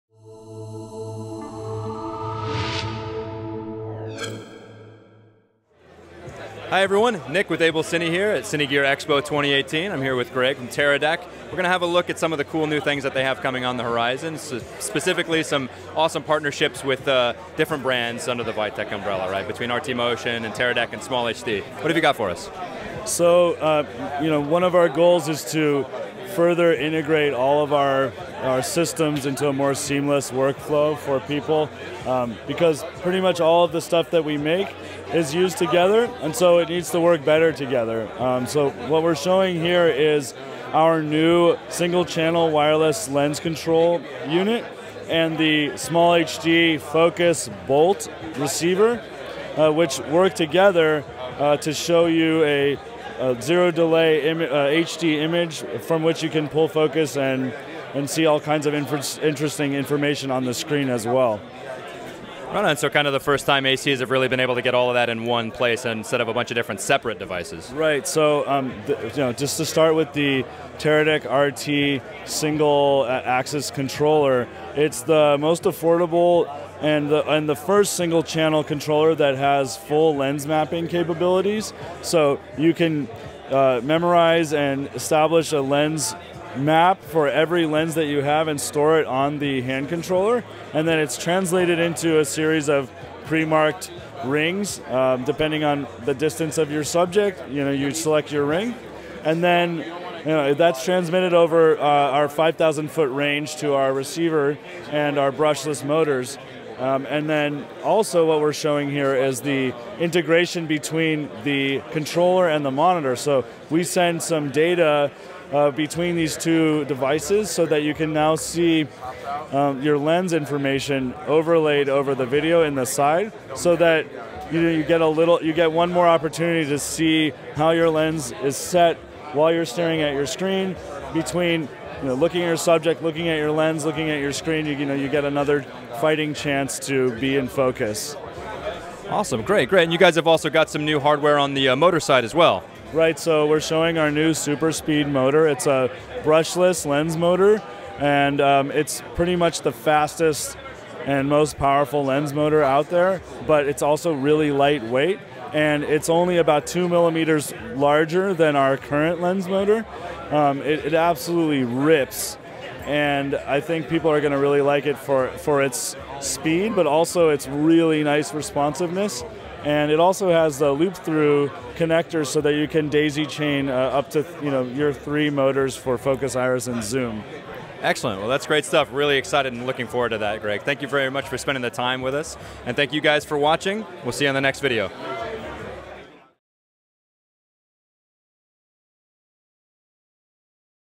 Cine Gear 2018: Teradek
Join Nic for an overview of what's new from Teradek for Cine Gear 2018. In the above video, Greg from Teradek shows off their tightly integrated approach to wireless on-set technology with the Teradek RT.